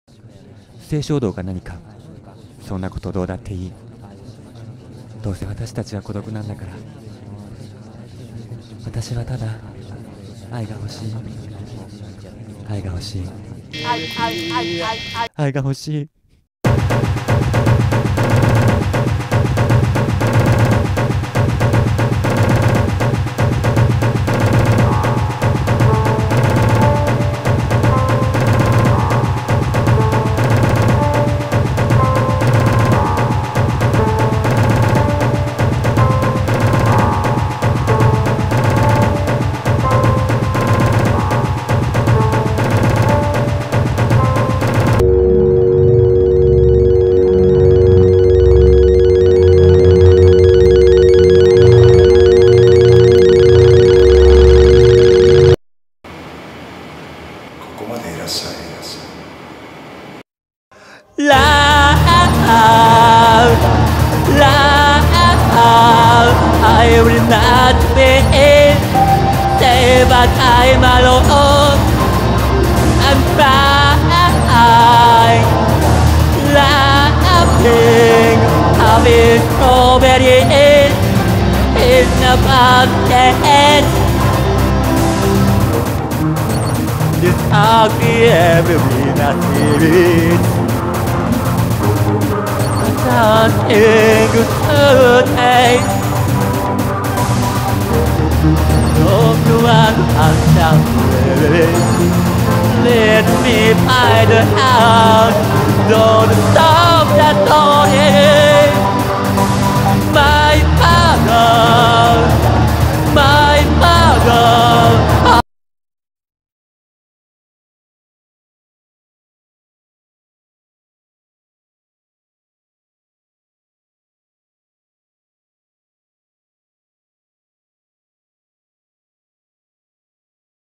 松山昂史 パフォーマンス "Categolibidorize" 予告映像 (Takashi Matsuyama performance "Categolibidorize" trailer)
“性衝動が何か、そんなことどうだって良い。
どうせ私たちは孤独なんだから。
私はただ、愛が欲しい、愛が欲しい、愛が欲しい”

名古屋の矢場町に位置するオルタナティブ・スペースspazio ritaにて、松山昂史のパフォーマンス作品と映像インスタレーション"Categolibidorize"が開催されます。2014年9月29日から10月2日までは映像インスタレーション、10月3日、4日にはパフォーマンスが行われます。
パフォーマンスは、今回も音楽、映像、身体の動きによって構成され、より身体性の強い作品に仕上がる予定です。特に、松山昂史自身の身体、経験を通し、ジェンダーやセクシャリティにフォーカスされたものとなるでしょう。
また、会場内には、東地雄一郎氏による写真展"いきうつし"が同時に開催されます。"いきうつし"は2013年に開かれた東地氏の個展。その個展を通し、プロジェクト"Categolibidorize"のコンセプトに、違った視点でアプローチしていただきます。ぜひ併せてお楽しみください。

映像インスタレーション"Categolibidorize"、東地雄一郎写真展"いきうつし"
2014年9月29日（月）～10月2日（木）
18:00 - 24:00
*要１ドリンク

松山昂史　パフォーマンス"Categolibidorize"
2014年10月3日（金）、10月4日（土）
20:00開演（19：30開場）
前売り：2500円（1ドリンク付き）
当日：3000円（1ドリンク付き）
*10月3日、4日は、パフォーマンス終了後に東地雄一郎写真展をご覧いただけます

会場：spazio rita（愛知県名古屋市中区栄5丁目26ー39 GS栄ビル B-1）

映像出演：
アンジェリカ
橋本知紗子

"What is my libido, what is your libido? These are trifling questions.
 Because we are alone in the end.
 I just want love, want love, want love"

 At an alternative space " spazio rita", Takashi Matsuyama's performance and video installation "Categolibidorize" will be held. You can find the installation from 2014 Sep. 29th to Oct. 2nd, and performance will be held Oct. 3rd and 4th.
 This performance is composed by music, video and move of body. Takashi's new performance focuses on gender or sexuality through his body and experience.
 At the same time, Yuichiro Higashiji's photo exhibition "Ikiutsushi" will be held at the same place. "Ikiutsushi" is Higashiji's solo exhibition in 2013. Through the exhibiiton, he will approach the concept of "Categolibidorize" by his own viewpoint. Hope you enjoy their works.

Video installation "Categolibidorize"、Yuichiro Higashiji photo exhibition "Ikiutsushi"
2014 Sep. 29th (Mon.) - Oct. 2nd (Thu.)
18:00 - 24:00
 *You need pay for 1 drink

 Takashi Matsuyama performance "Categolibidorize"
 2014 Oct. 3rd (Fri.) , Oct. 4th (Sat.)
 20:00 start (19：30open)
 Advance : 2500yen (with 1 drink)
 Doors : 3000yen (with 1 drink)
 *You can enjoy Yuichiro Higashiji's photo exhibition after performance

Booking for performance or inquiries, please mail this adress.

Cast in video work:
Angelica
Chisako Hashimoto